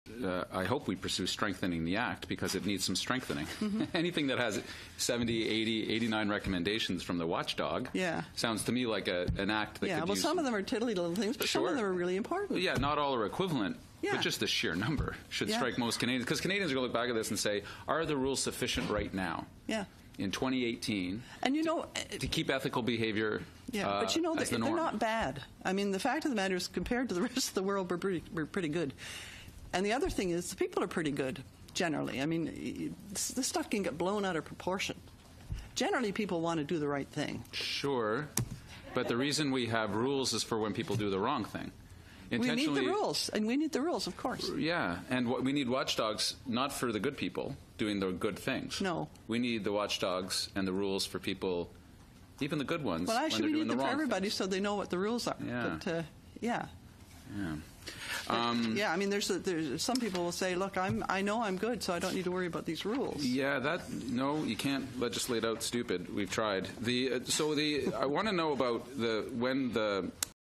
You Can't Legislate Out Stupid, We've Tried
While discussing strengthening the Ethics rules Nathan Cullen reacts to Mary Dawson's comment "generally people want to do the right thing".